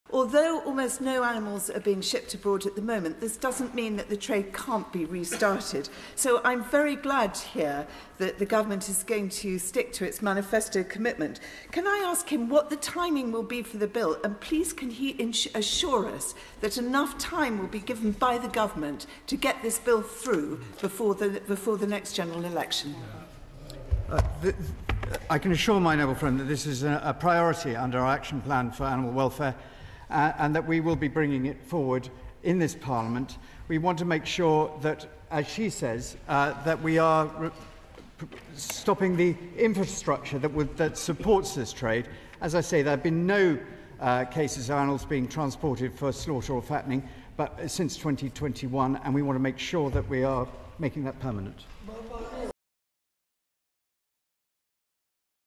Lords questions on banning live animal exports
Baroness Hodgson of Abinger pressed the government on its plans to ban the export of live animals for slaughter and fattening.

Lords questions takes place every sitting Monday to Thursday. Watch live or catch up on Parliament TV